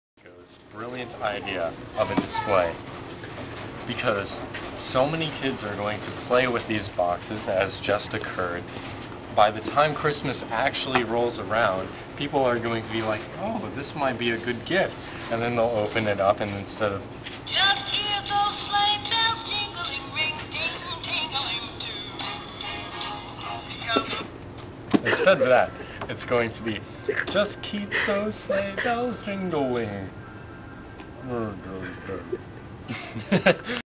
Kinkoland Adventures!!! Singing Box TIIIIMMMME!!! Part one.
the box sings. greg dances.